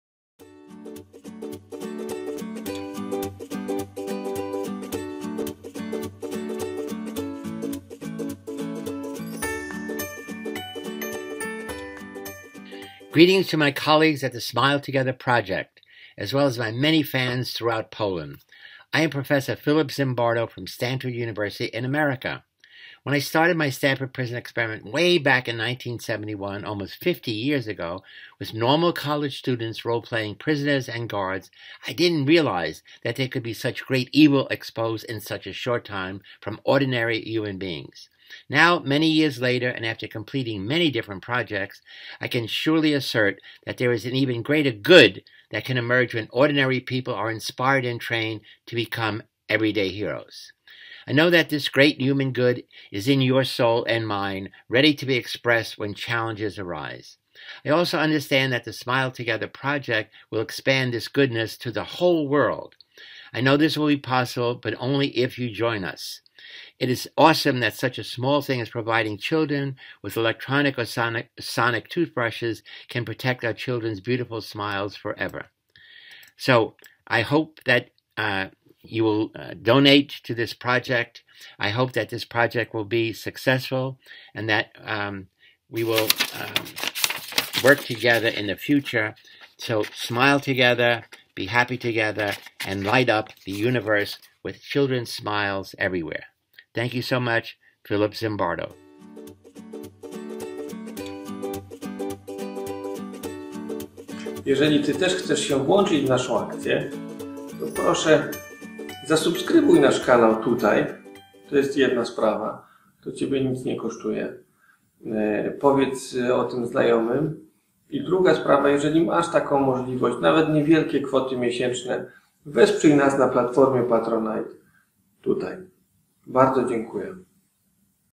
Philip Zimbardo about Smile Together
Pan Profesor Philip Zimbardo jest osobą, której nie trzeba przedstawiać bo nawet jeżeli ktoś nie zna jego nazwiska to z pewnością słyszał o eksperymencie więziennym.

Nie wszystkim jednak wiadomo, że Profesor Zimbardo jest założycielem Heroic Imagination Project.

Gorąco zachęcam do odwiedzenia strony i włączenia się w działania, które sprawiają, że każdy z nas może zostać prawdziwym bohaterem.

Jeżeli Pan Profesor jest dla Ciebie autorytetem i TY też chcesz zostać bohaterem dnia codziennego to możesz to zrobić jako fundator akcji Smile Together.

Panie Profesorze, bardzo bardzo dziękuję Panu za wsparcie!!! To niesamowite, że taki autorytet jak Pan, zechciał wesprzeć naszą akcję!!!

Wierzę, że to będzie zachęta dla każdego, żeby się do nas dołączył i żeby uśmiechy wszystkich dzieci, tak jak Pan to ujął, rozświetliły cały wszechświat!